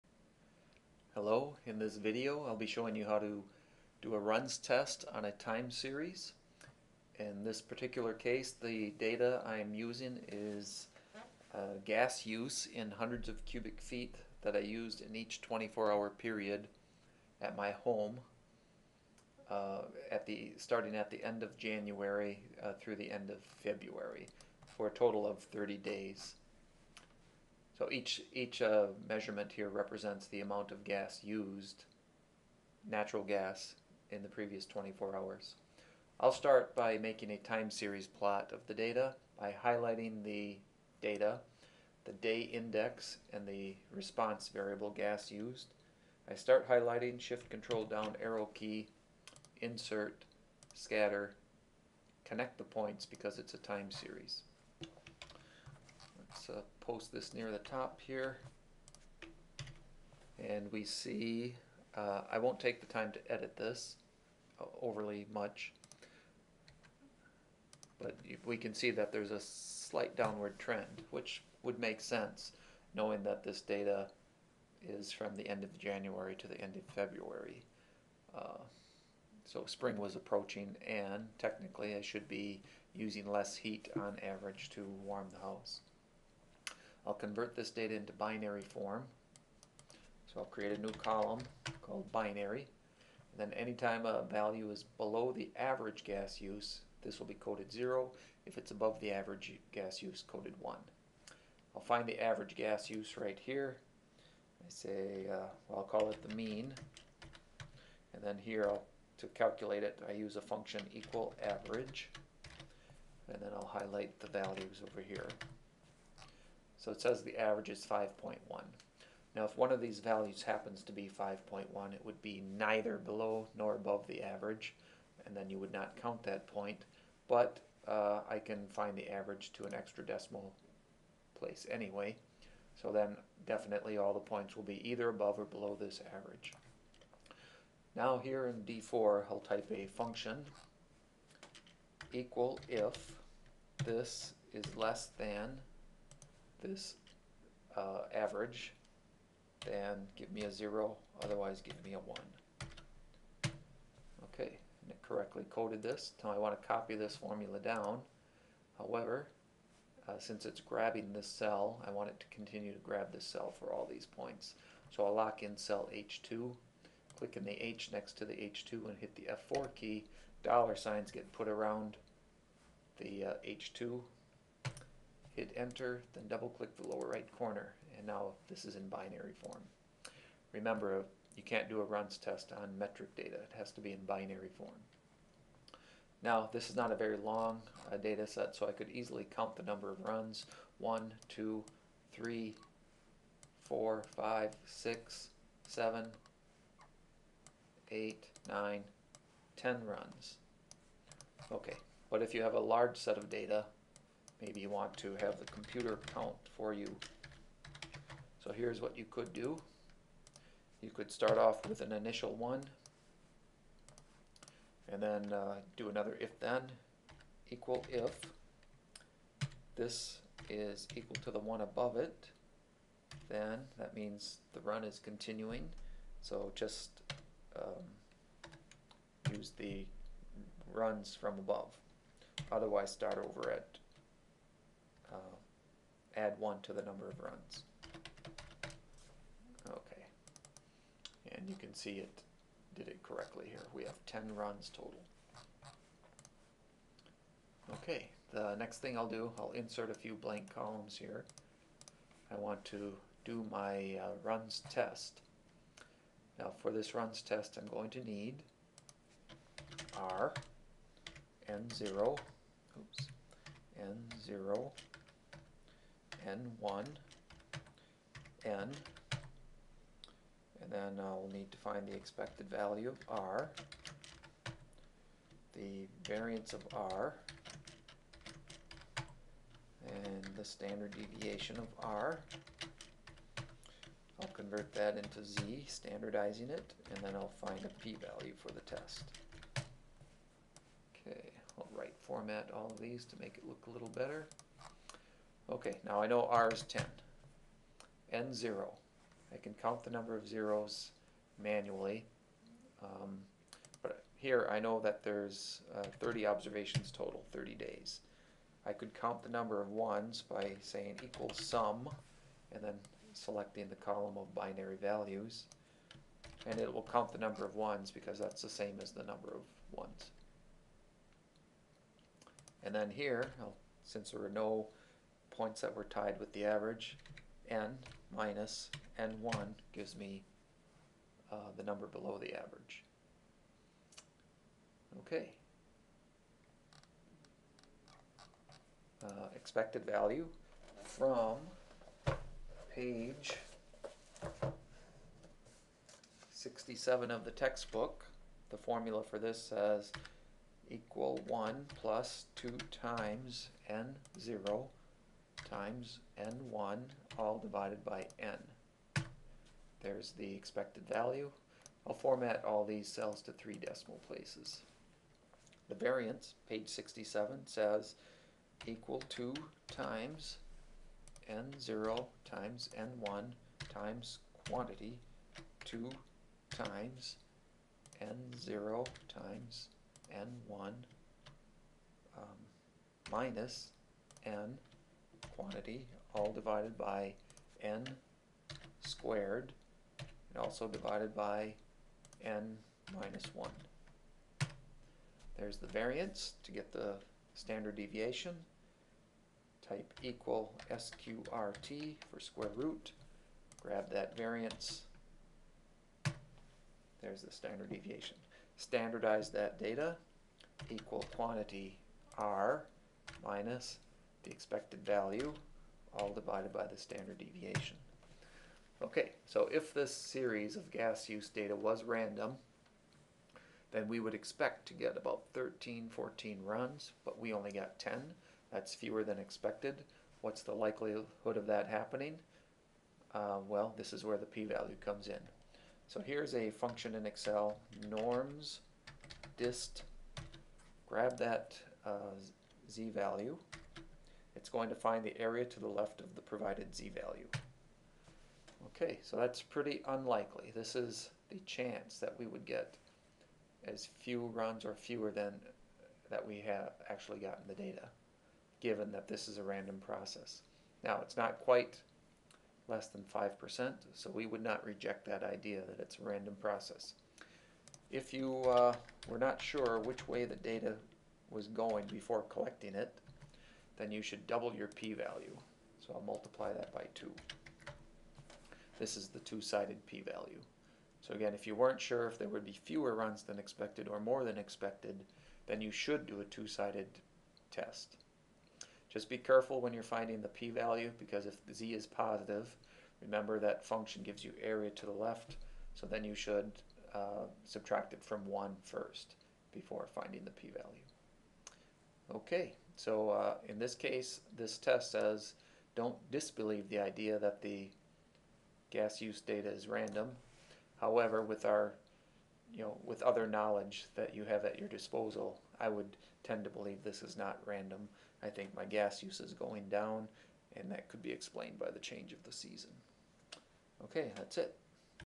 How to Do a Runs Test in MS Excel 2007
This video shows you how to do a runs test in the basic package of MS Excel 2007 (i.e, without an Add-In).  The purpose of a runs test is to determine if a set of binary data collected over time is random or not.  The data I used represents the natural gas used in my home on a daily basis over a period of 30 days - starting near the end of January and ending near the end of February.  The units of gas are in 100s of cubic feet.